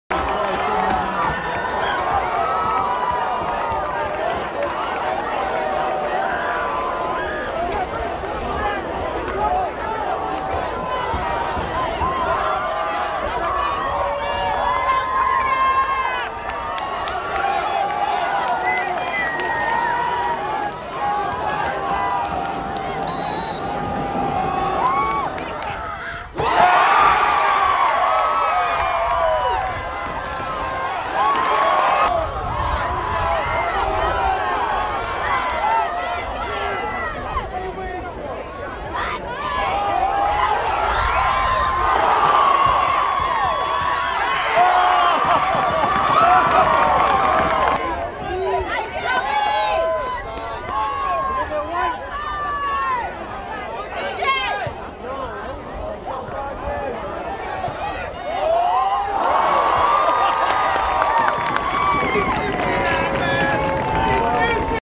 goalie stops 10/25/14
Goalies never get enough credit, but Casey Henry goalie for Daniel Hand High School under duress made some incredible saves during Saturday nights SCC Playoff game against Guilford HS Hand won 1-0 in full time!